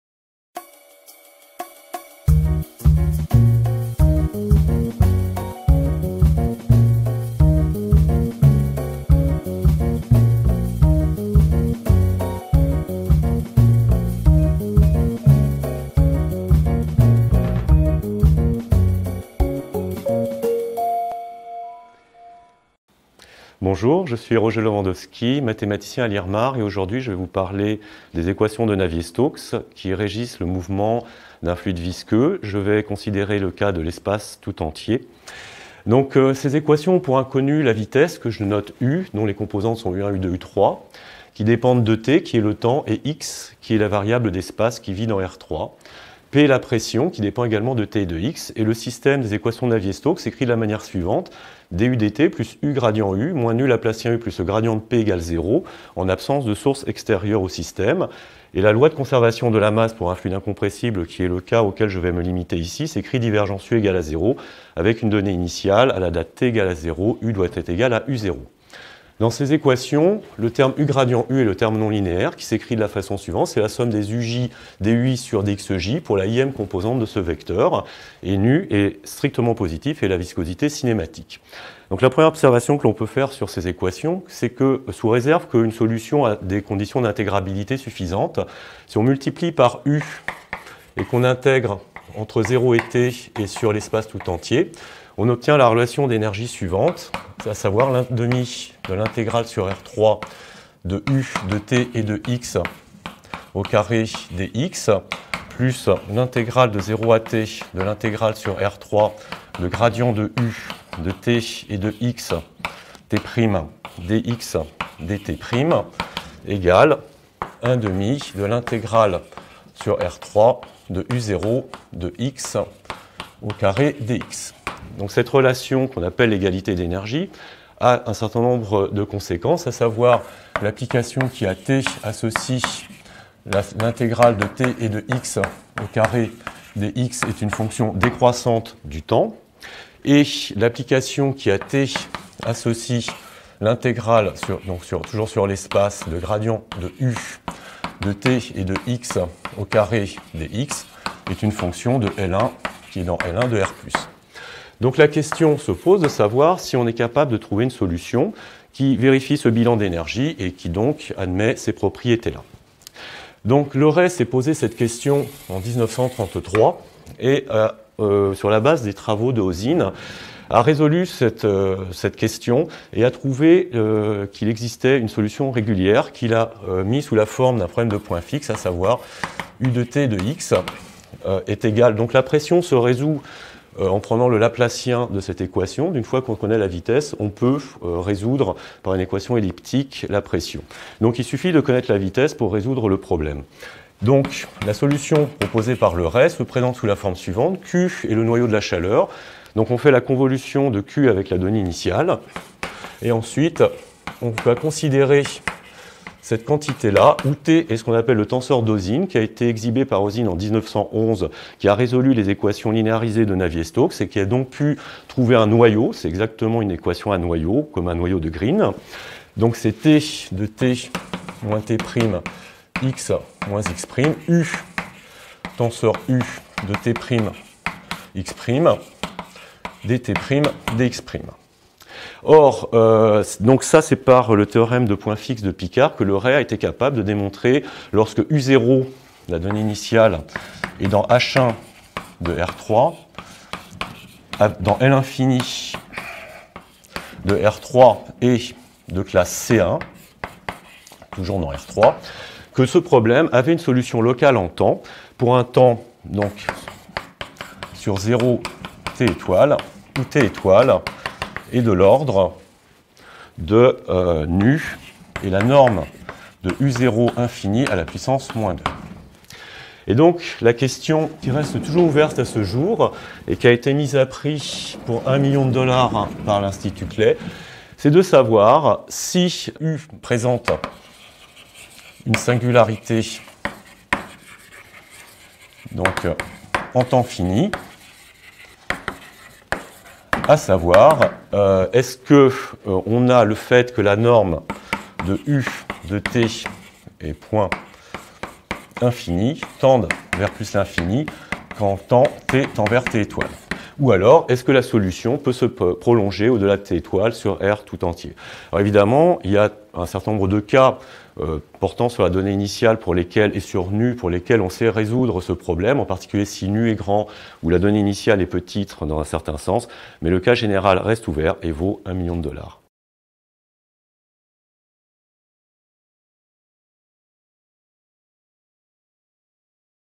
Roger Lewandowski - Pour une poignée de dollars
Depuis Euler jusqu’à Navier, on s’est attaché à élaborer les équations qui régissent le mouvement des fluides visqueux. Stokes leur donna leur lettres de noblesse : ce sont aujourd’hui les équations de Navier-Stokes. Au début du 20ème siècle, Oseen ouvre la voie en s’attaquant au cas linéarisé, mais c’est Leray en 1934, un jeune mathématicien alors  Rennais, qui dans un travail visionnaire décrit en détail la structure du système dans le cas incompressible. Il montre l’existence d’une unique solution régulière, locale en temps. Incapable de montrer que cette solution se prolonge pour tout temps dans le cas général, de dépit,  il  introduit la notion de solution turbulente, aujourd’hui solution faible, et montre l’existence d’une solution turbulente globale en temps. Mais reste ouvert le problème de savoir si la solution régulière peut développer une singularité en temps fini ou pas, ce qui est étroitement lié à la structure de la solution turbulente. Ce problème, mis à prix 1 million de dollars par la fondation Clay en 1991, est des six problèmes de mathématiques les plus difficiles aujourd’hui non résolus.